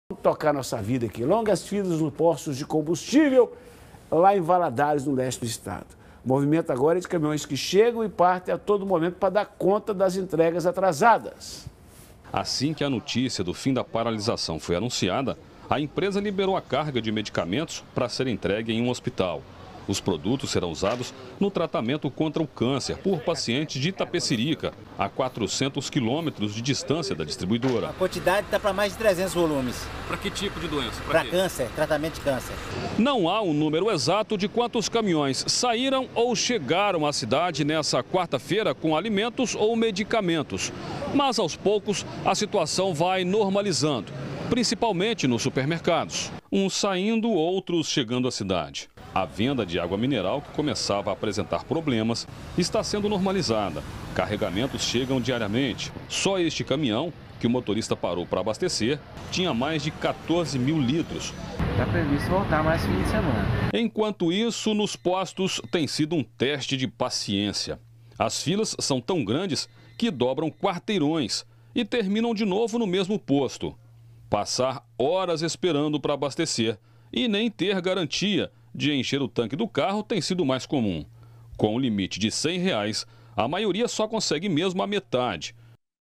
Movimento de caminhoneiros aumenta no interior de MG
Longas filas nos postos de combustíveis, de Governador Valadares, no leste do Estado. O movimento agora é de caminhões que chegam e partem a todo o momento para dar conta das entregas atrasadas.